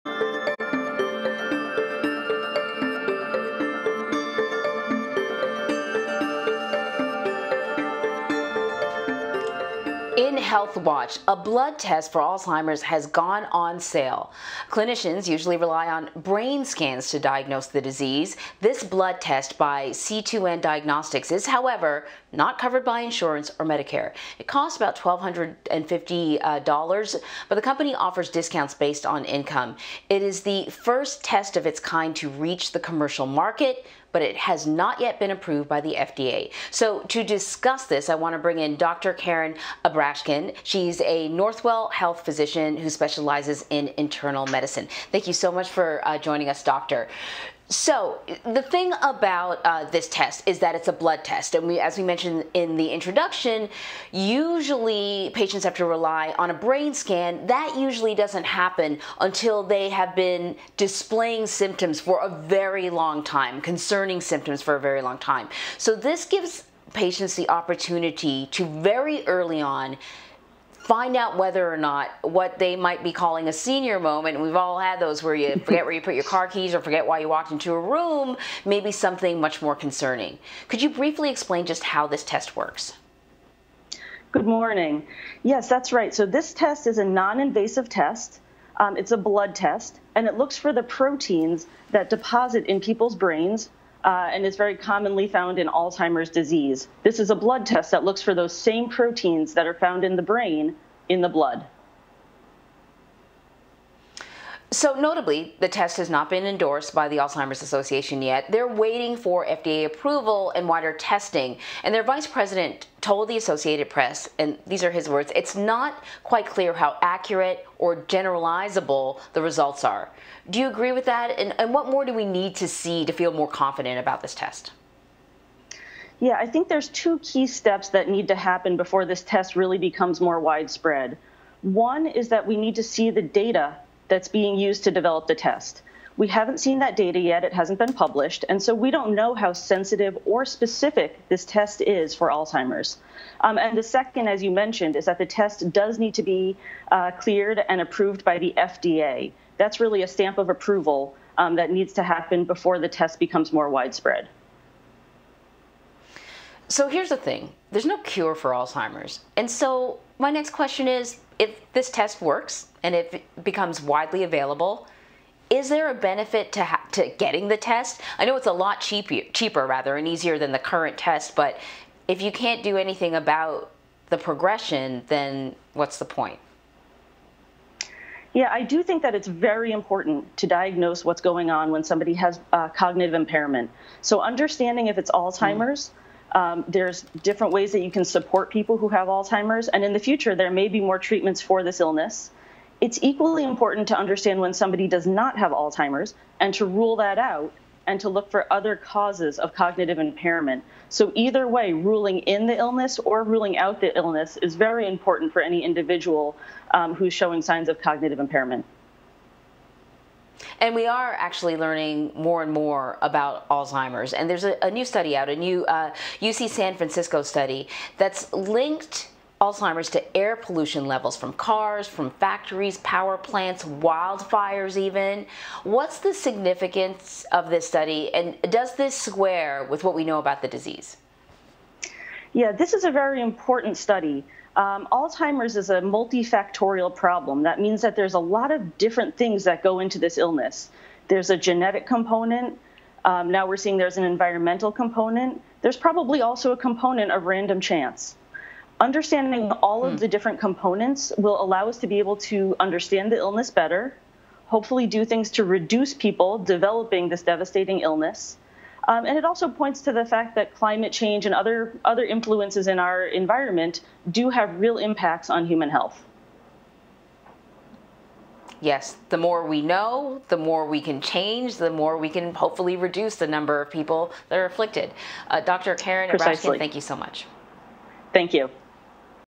Blood test for Alzheimer's goes on sale without FDA approval
St. Louis-based C2N Diagnostics has released a blood test that it says can detect Alzheimer's disease. The test is not yet FDA approved, and is not covered by insurance, costing up to $1,250. Northwell Health internal medicine specialist Dr. Karen Abrashkin joins CBSN's Anne-Marie Green to talk about the latest development in the fight against Alzheimer's, which affects nearly 6 million Americans.